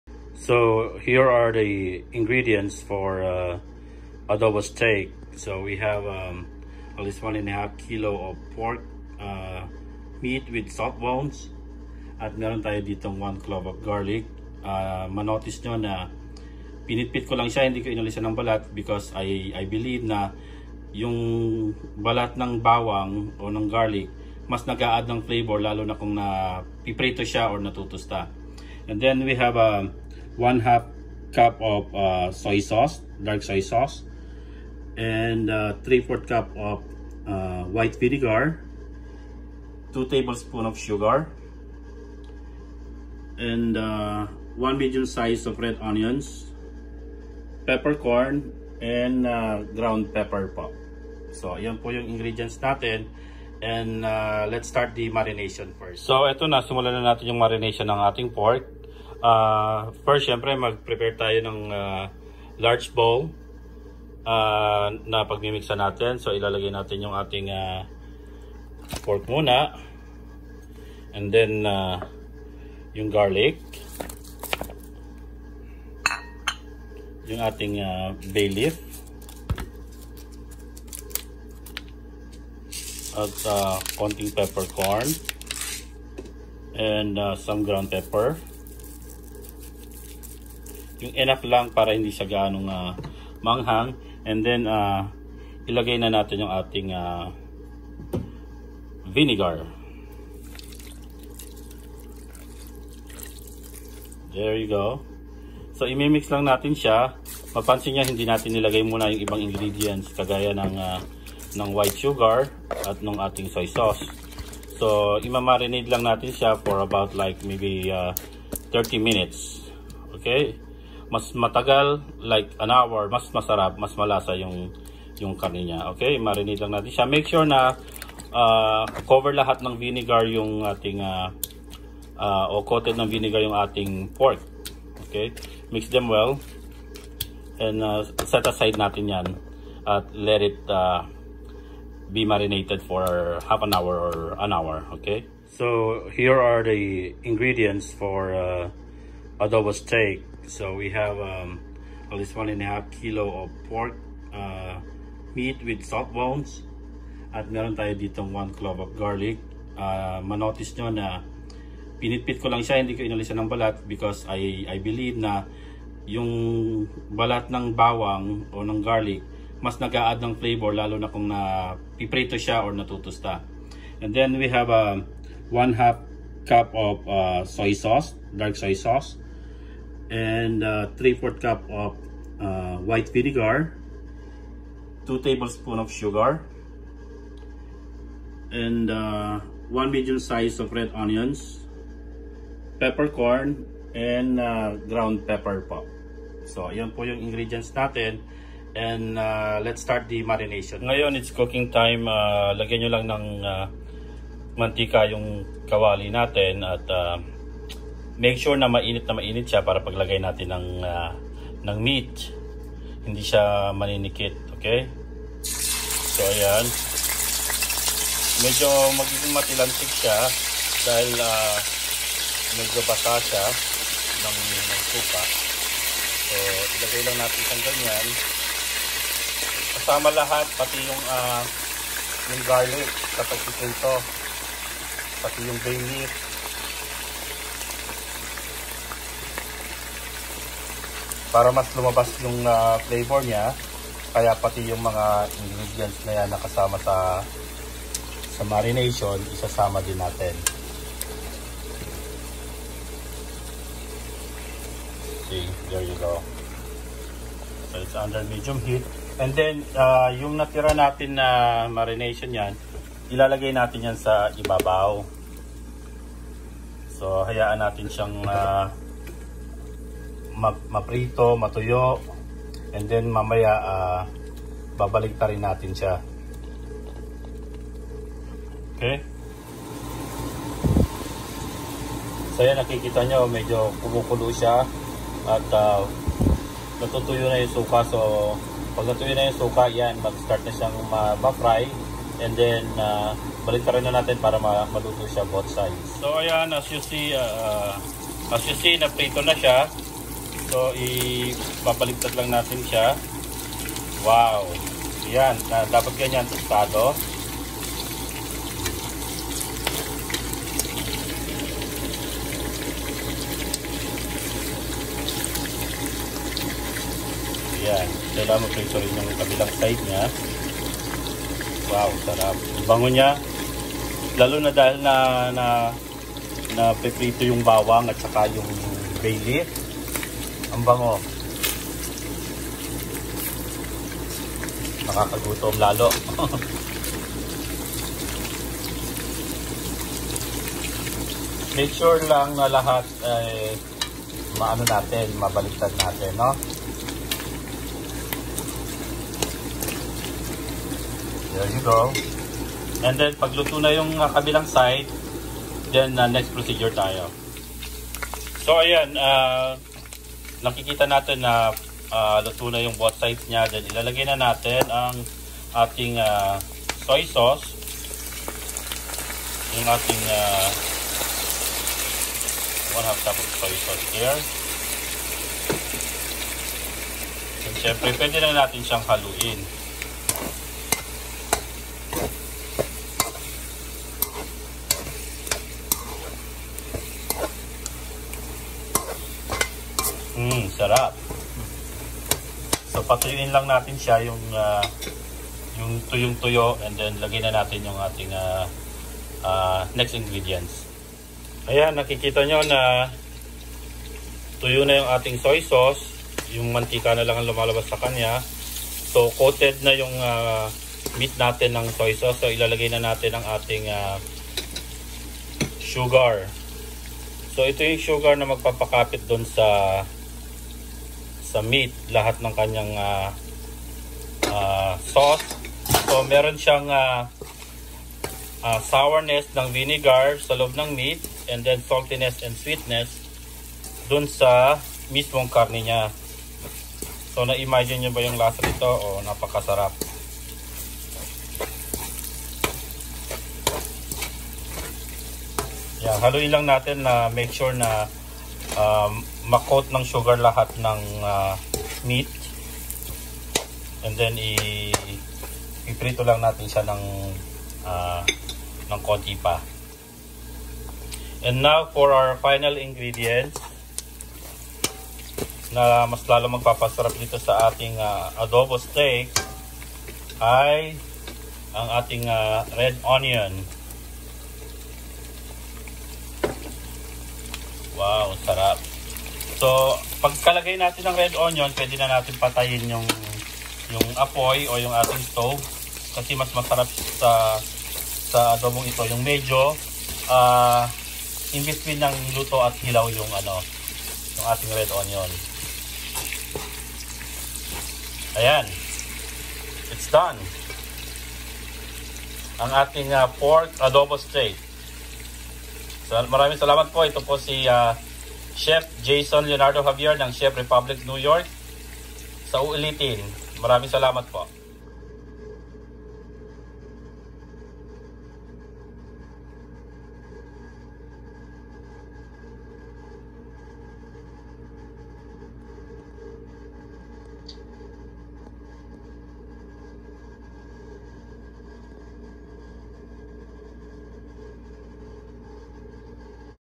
Cooking Pork Adobo Steak with Chef Jayson!
Pork Adobo Steak is a modified traditional filipino Saucy Adobo. Also called “Adobong Matanda” or Dried Adobo in Taytay, Rizal.